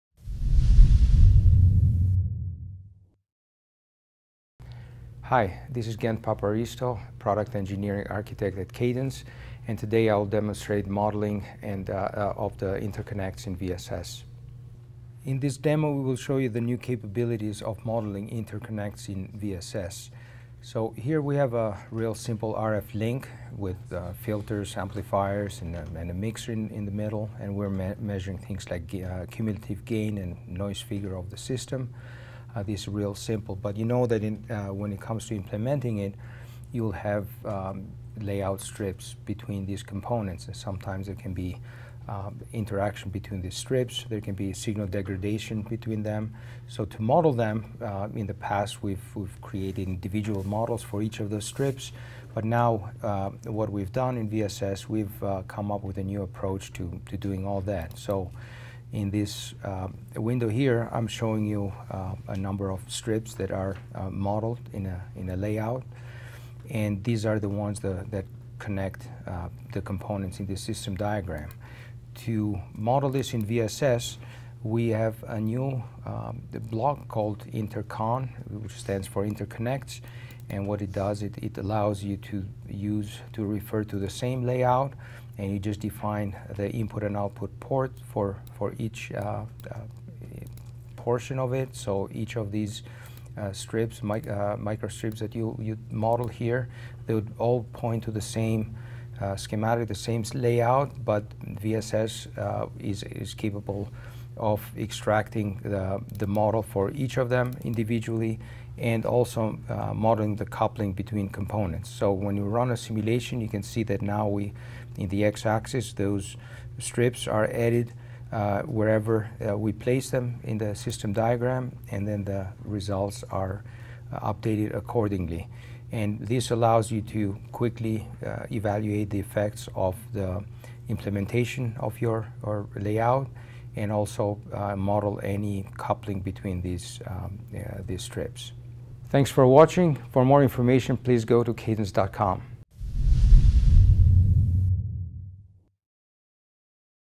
Modeling Physical Layout in RF System-Level Design with Cadence VSS Software
See the new capabilities of behavior modeling interconnects in Cadence AWR Visual System Simulator (VSS) RF/wireless communications and radar system design software.